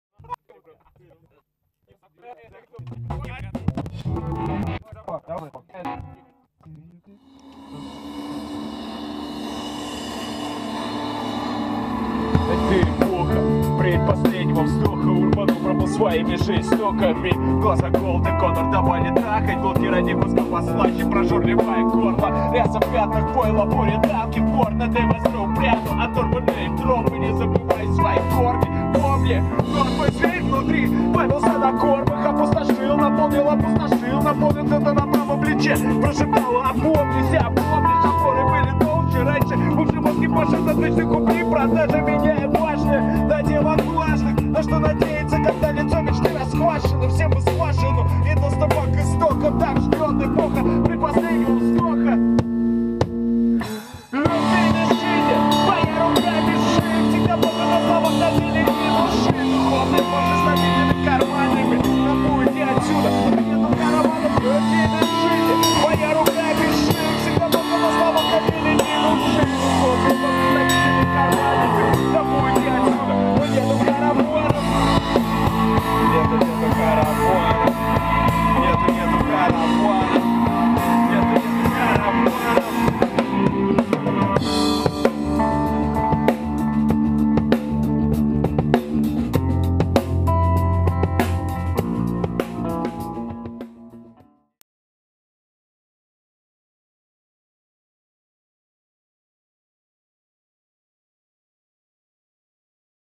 Indiana and Franklin Promo Video 2
Indiana Franklin - группа друзей из Балашихи, которых объединила любовь к музыке.
Творчество группы сочетает в себе энергию живой музыки и дерзкий речитатив в лучших традициях классического хип-хопа. Лирика группы затрагивает актуальные проблемы нашего времени в сознании каждого человека и общества в целом.